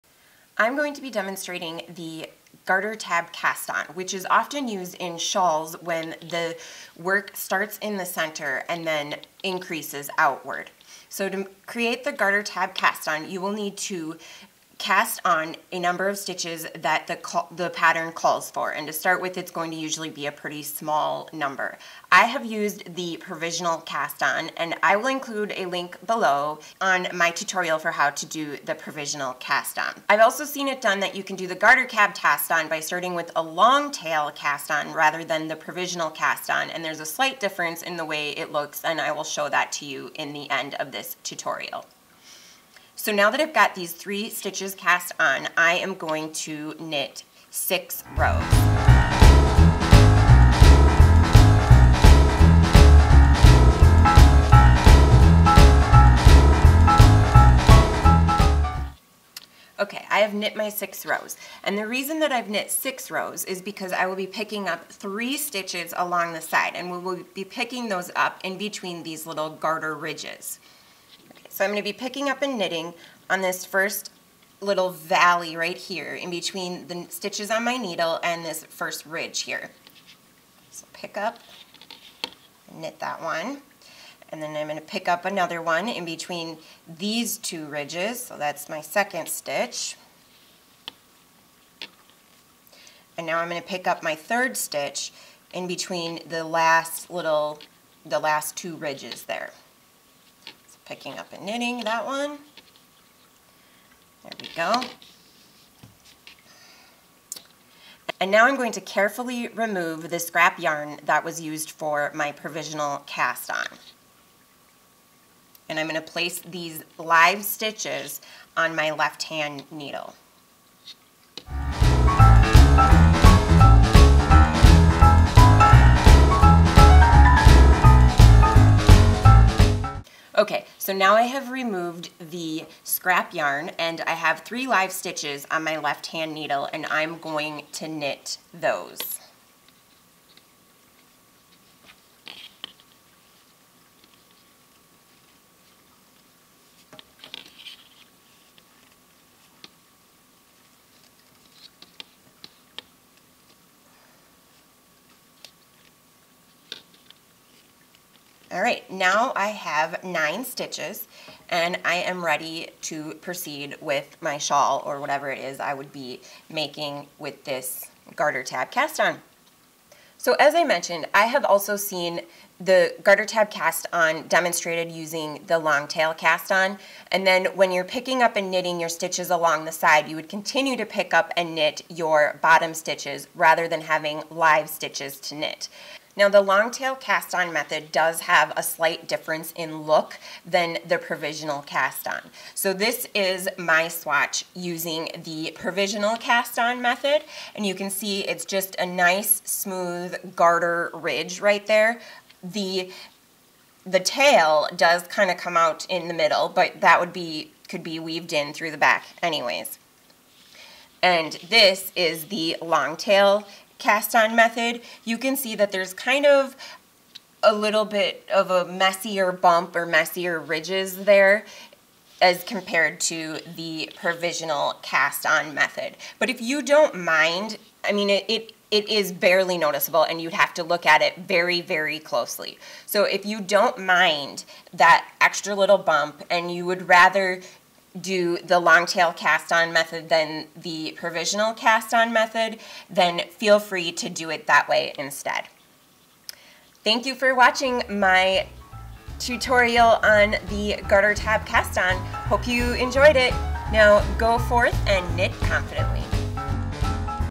Tutorial for Garter Tab Cast On
In this tutorial I explain how to do a garter tab cast on, which is often used for shawls that start in the center and increase outward.

You may also find my pick up and knit and provisional cast on tutorials helpful.

Reference

Editors of Vogue Knitting Magazine, Vogue Knitting the Ultimate Knitting Book. New York: Sixth&Spring Books, 2018.